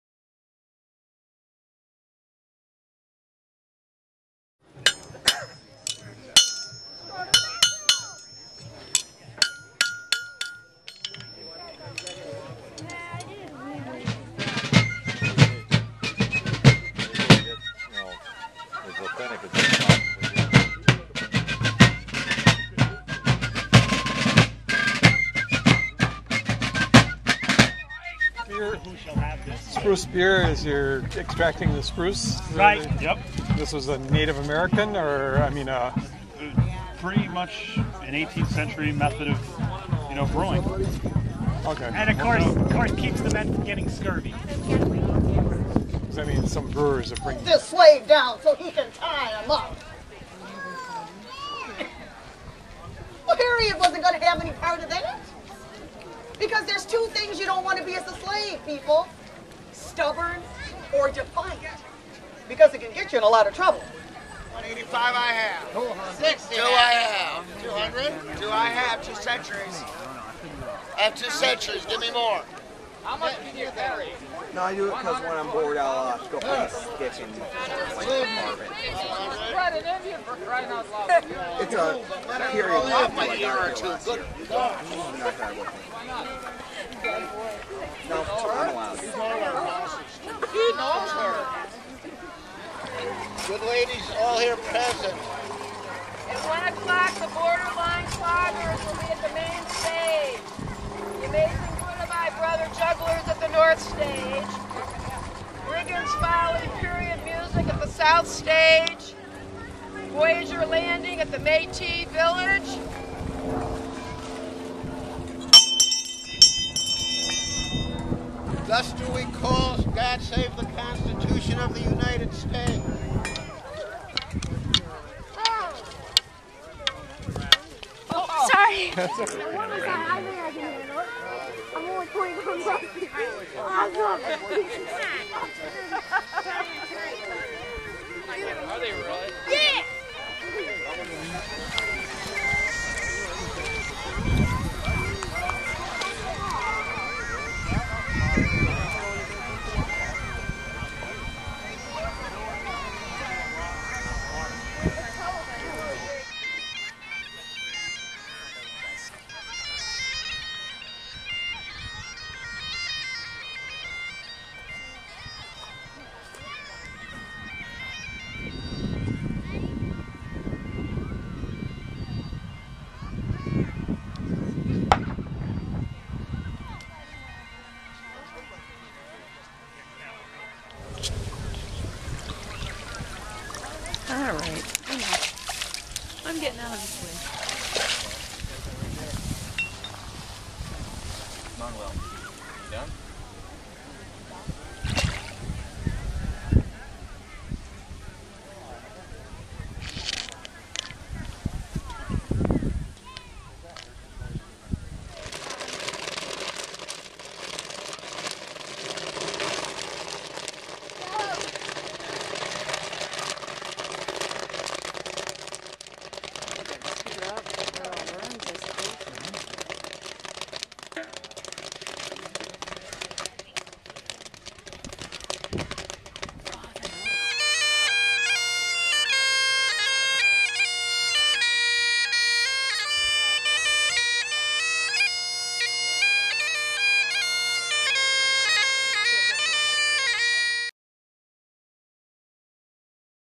Trail of History
October 17, 2001. The Trail of History at Glacial Park, Ringwood, IL